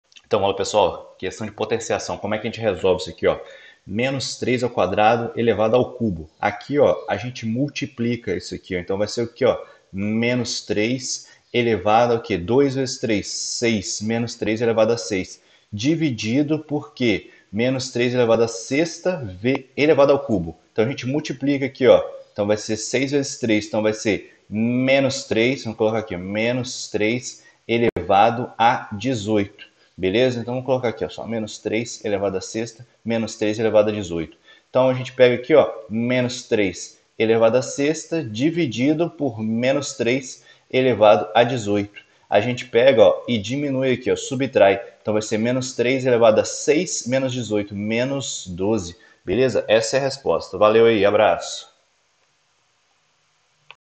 POTENCIAÇÃO
A potenciação ou exponenciação é a operação matemática que representa a multiplicação de fatores iguais. Ou seja, usamos a potenciação quando um número é multiplicado por ele mesmo várias vezes.

Lembre-se as resoluções estão em video no canal.